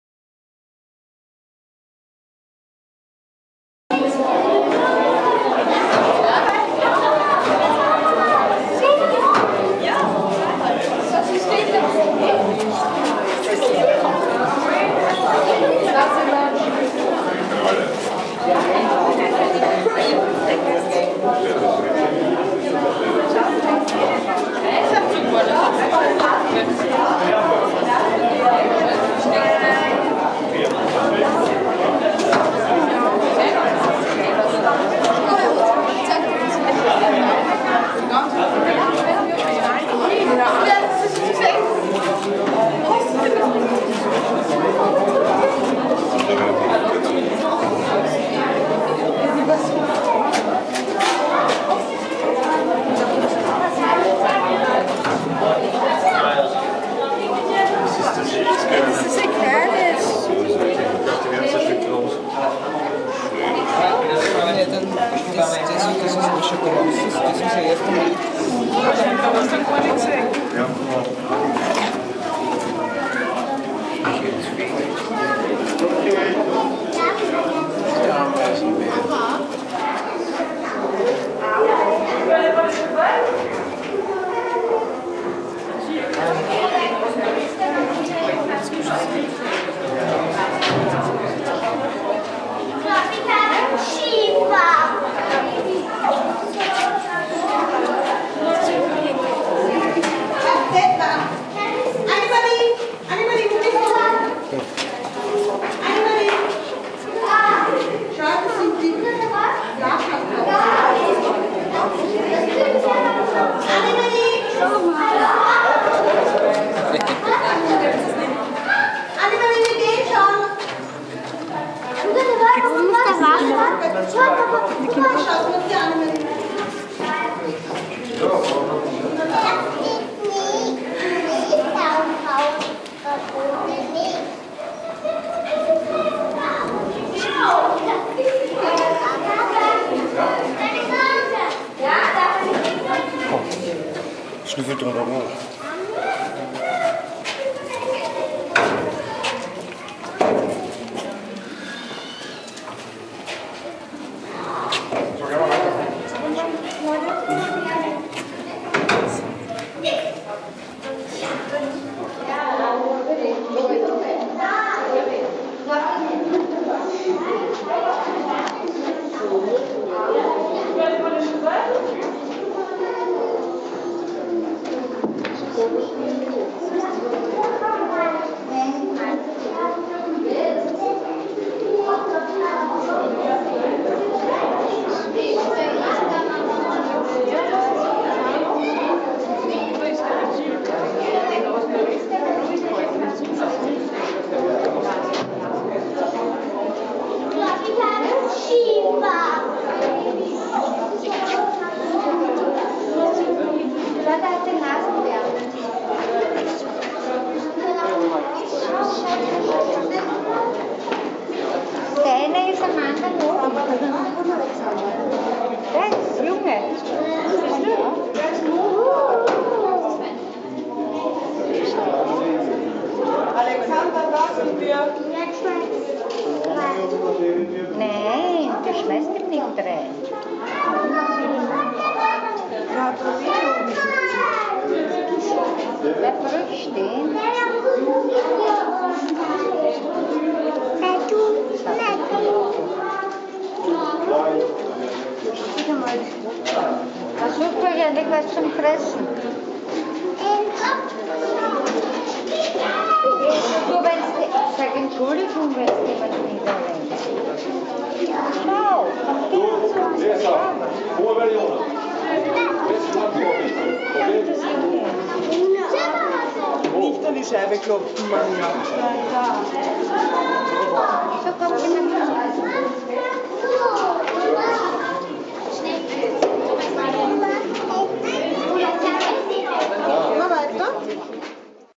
Hombrecito wird größer und größer
noch wird er getragen.
im vergleich zur kleinen Mocoa ist er ja schon riesig